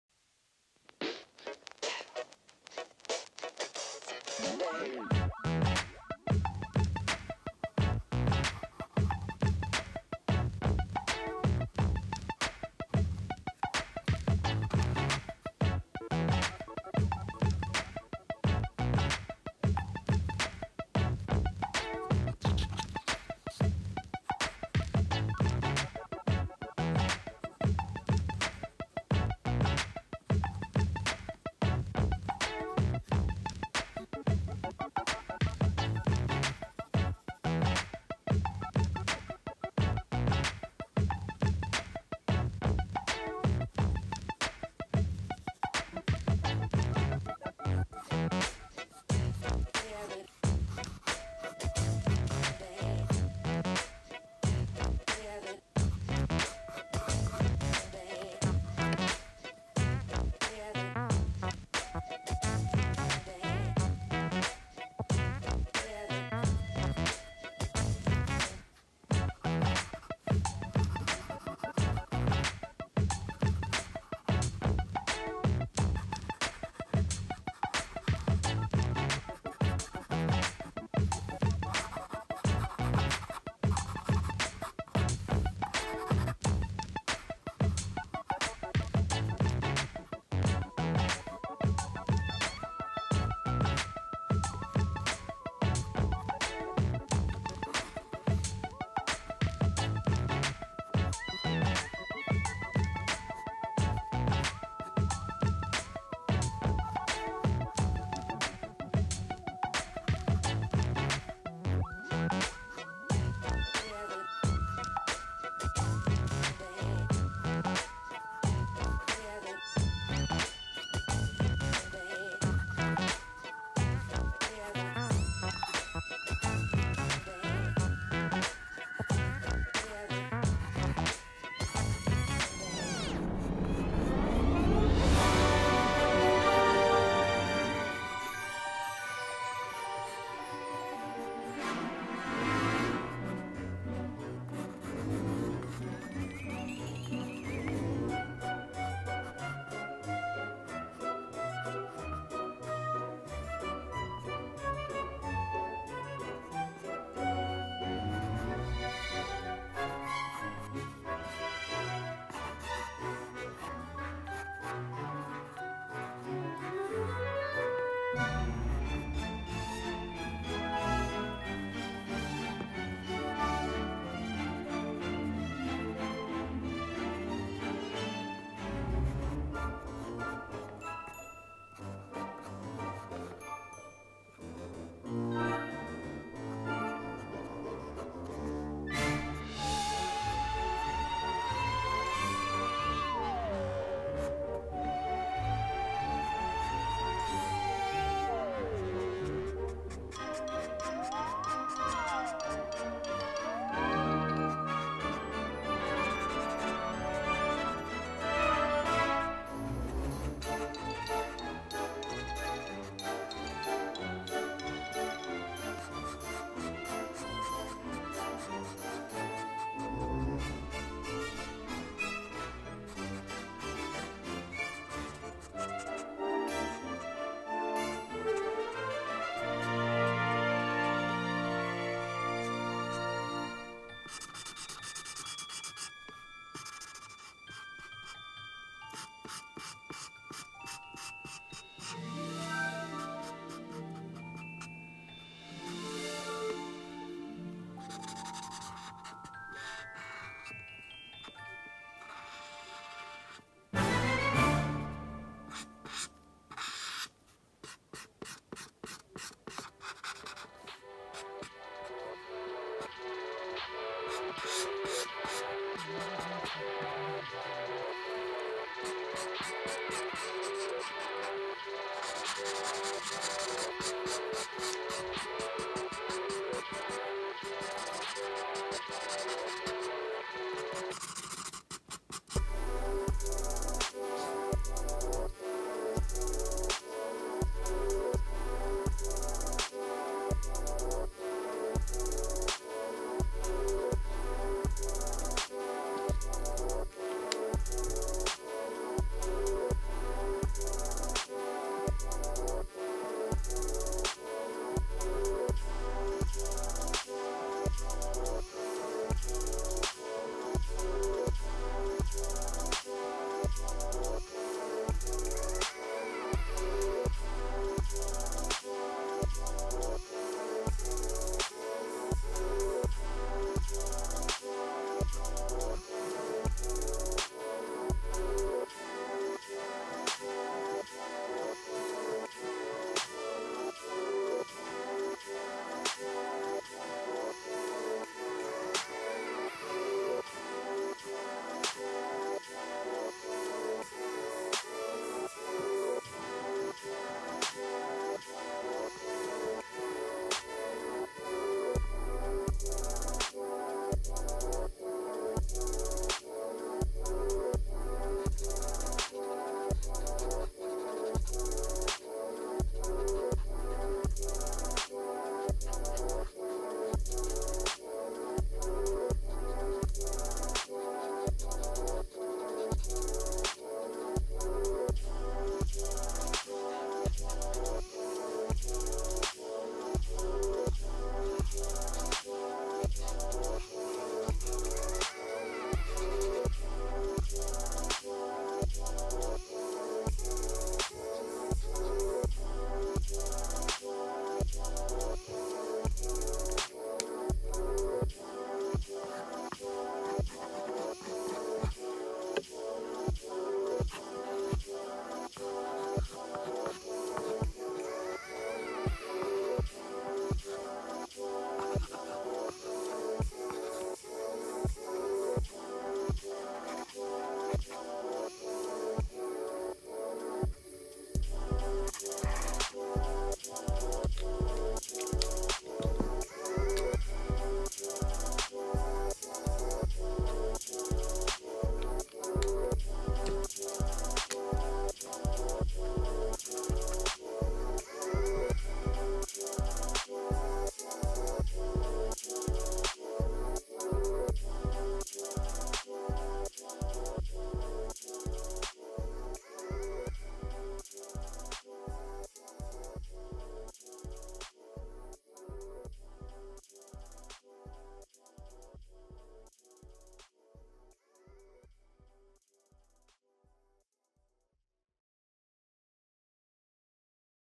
Ursula from Disney's 'The Little Mermaid' Sketch To Colour Illustration | Hayden Williams
I've been creating a lot of illustrations from 'The Little Mermaid'  this year in honour of the films 30th anniversary. Here is some footage of me creating a new Ursula illustration. I hope you all enjoy!